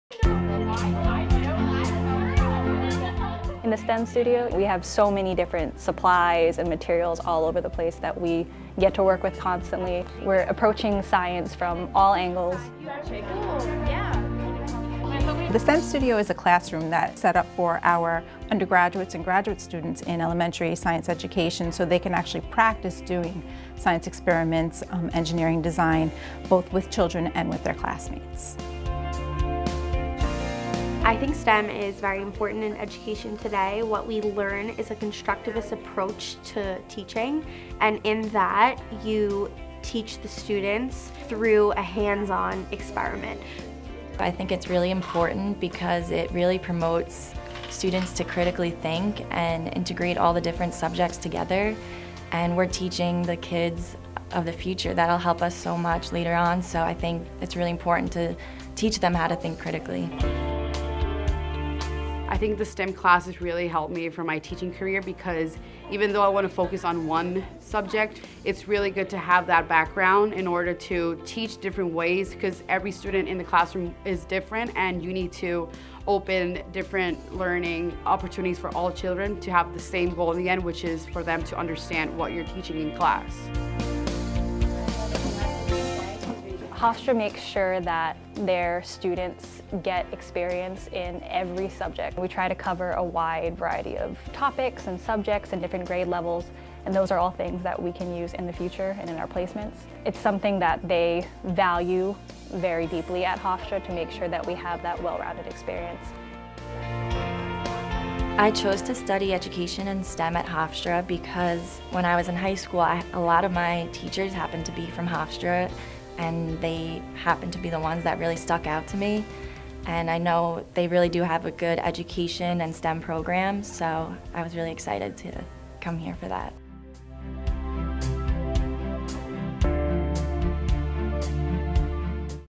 STEM Studio
The Hofstra University STEM Studio brings teachers and students together, on the campus of Hofstra University, to participate in a teaching and learning laboratory focused on a problem-based approach to teaching. The studio provides math, science and engineering design challenges that integrate literacy, social studies, multiculturalism, and the arts.